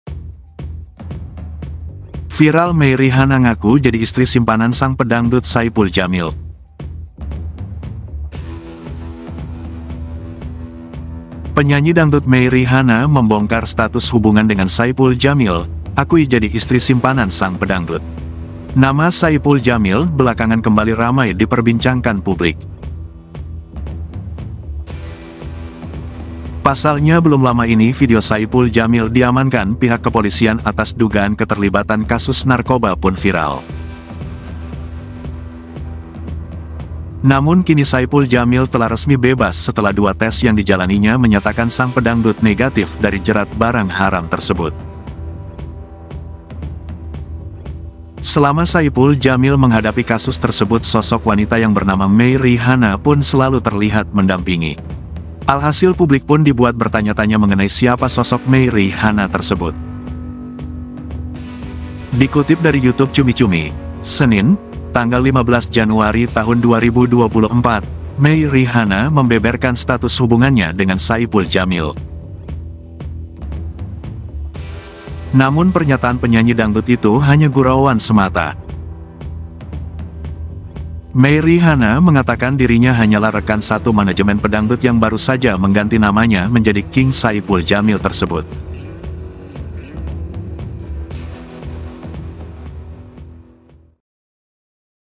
Viral Mey Ri Hana Ngaku Jadi Istri Simpanan sang Pedangdut Saipul Jamil
Penyanyi dangdut Mey Ri Hana membongkar status hubungan dengan Saipul Jamil, akui jadi istri simpanan sang pedangdut. Nama Saipul Jamil belakangan kembali ramai diperbincangkan publik.

Pasalnya belum lama ini video Saipul Jamil diamankan pihak kepolisian atas dugaan keterlibatan kasus narkoba pun viral.